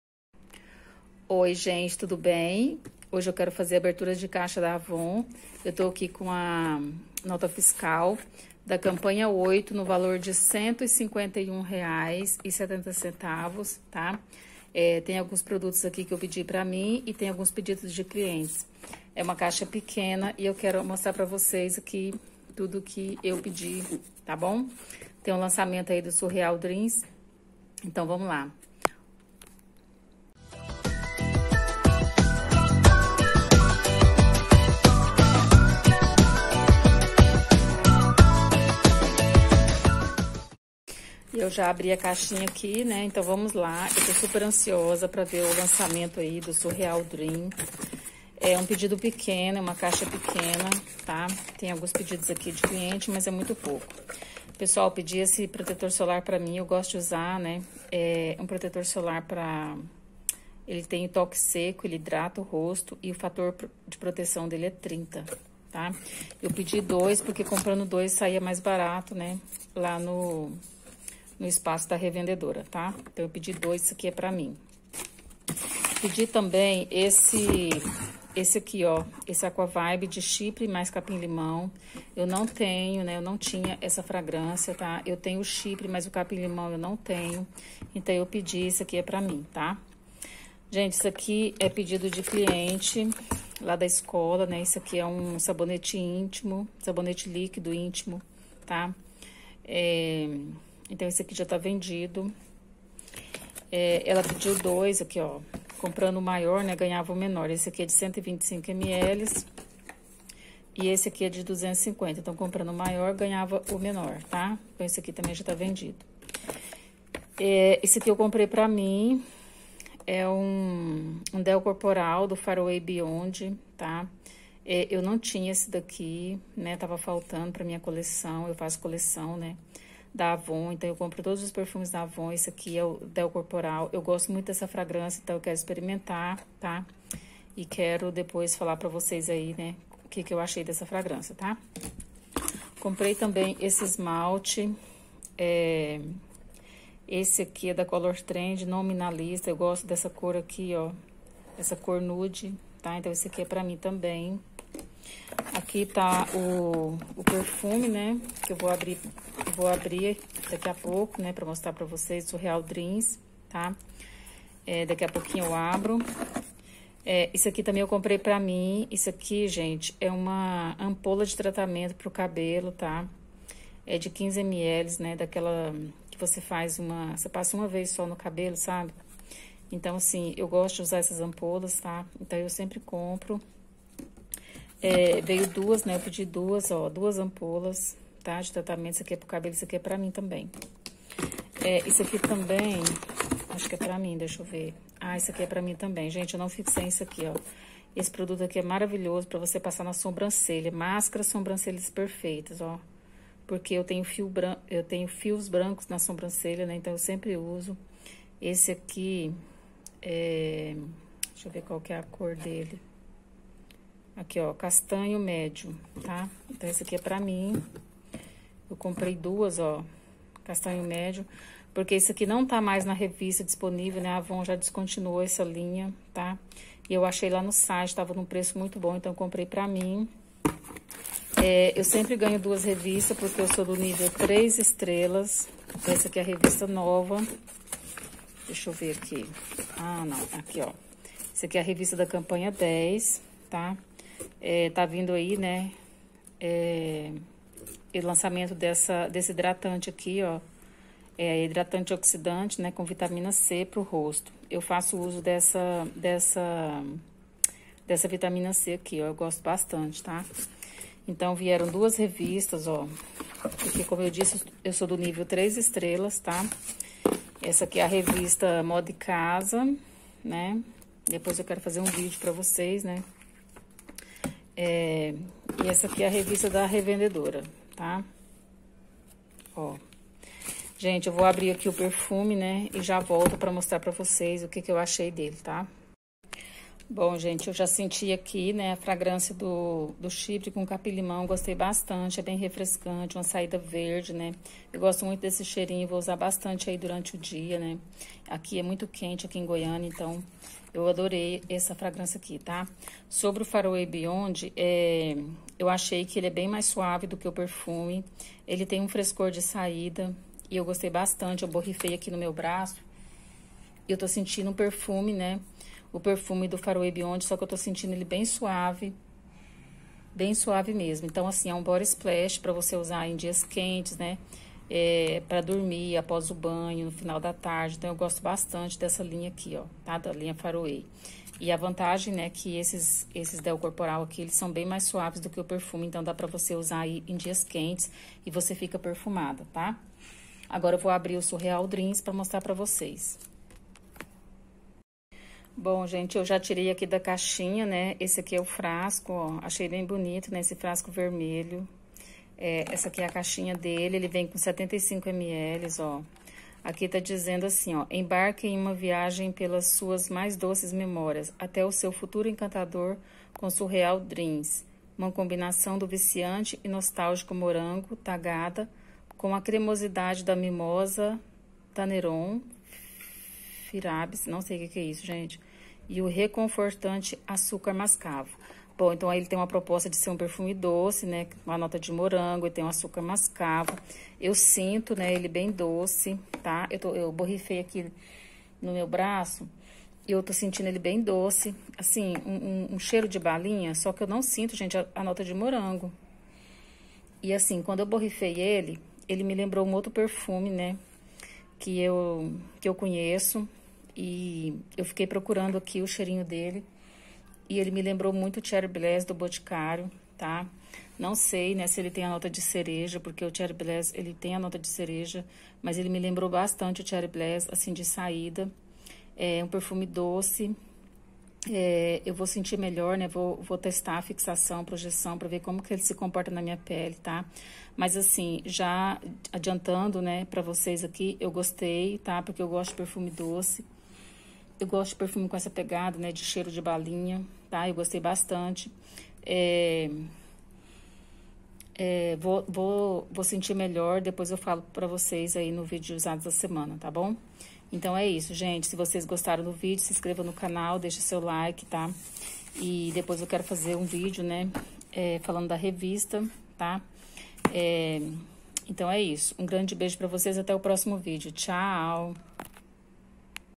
ABERTURA DE CAIXA AVON DA CAMP. 08 COM LANÇAMENTO DO PERFUME SURREAL DREAMS
Chegou a caixa da campanha 08 da AVON 😀👏👏👏👏 bora conferir esse pedido? 😊😜
Vem comigo 🤗☺️

Pedi o perfume Surreal dreams 🍓 mais um lançamento da AVON para a minha coleção!!! 🥰
Você já conhece? Me conta aí que eu vou adorar saber. 😃

Para comprar Natura com desconto de 25% use o cupom abaixo 👇👇👇👇
Válido até o dia 31 de março de 2022.
cupom MARCO